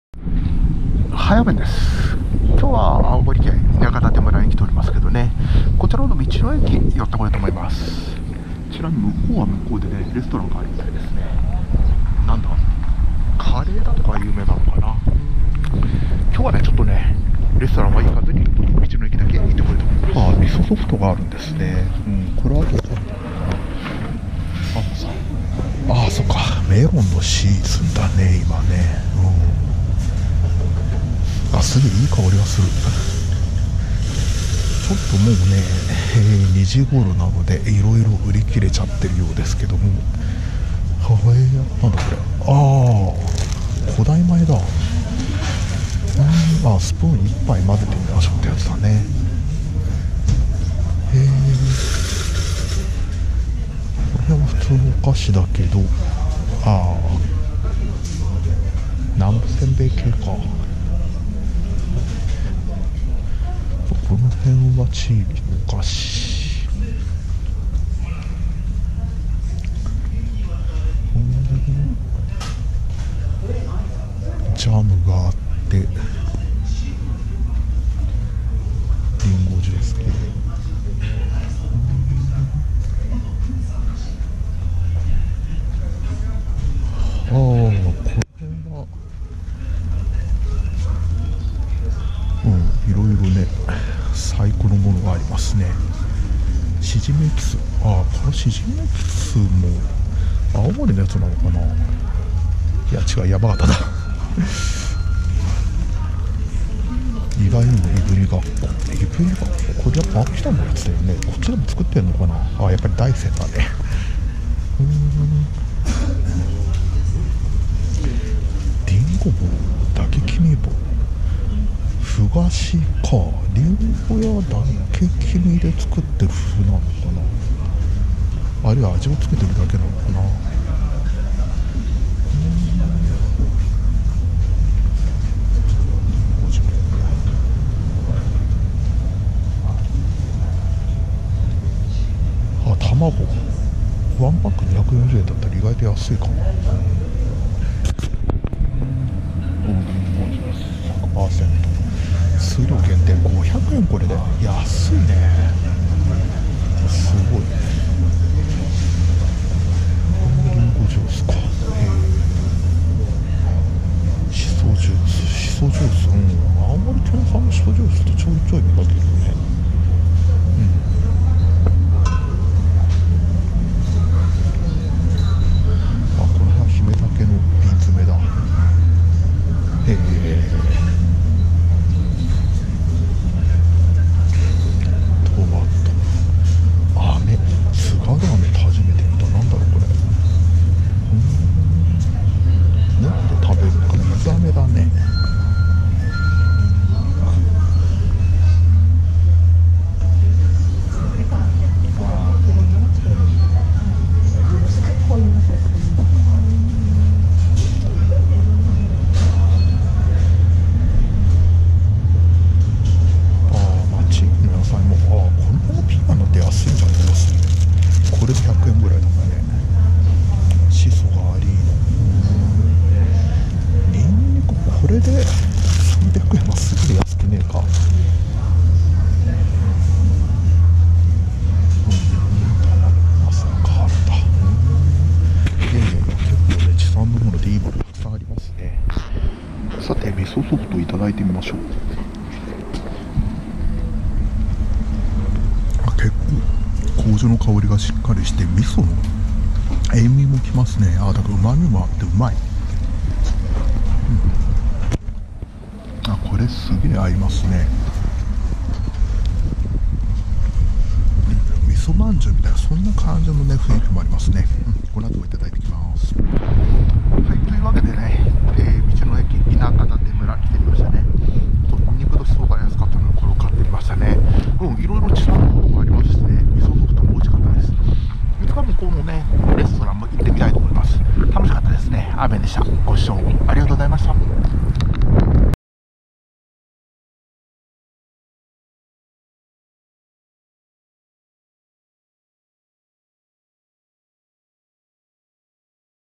道の駅田舎館
青森県の道の駅田舎館に行ってきました。